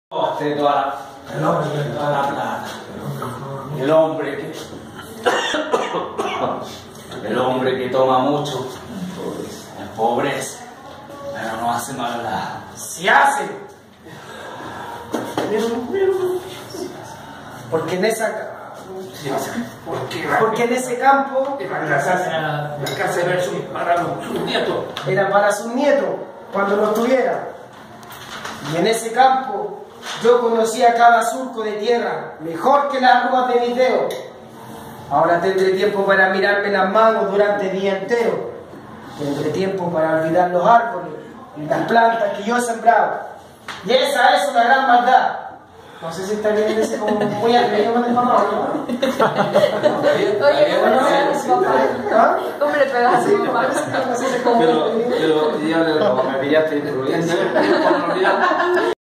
El cheruve: Obra de teatro mitologica y constumbrista mapuche de Alejandro Sieveking
ensayo de una obra de teatro en casa de la cultura de quinta normal. Participantes y actores de la Obra : alejandro torres Aravena; Barbara Salazar; Carlos Alejandro Villablanca Villarroel; Jose Anibal Abarca Navarrete; Julio Fuentes Manriquez; Linda Flores Miñoz; Maria Ines Toro; Nicole Cruz; romina Ibarra; Wladimir Michael Araya Oportus;  Franco Gonzalez Fortunatti;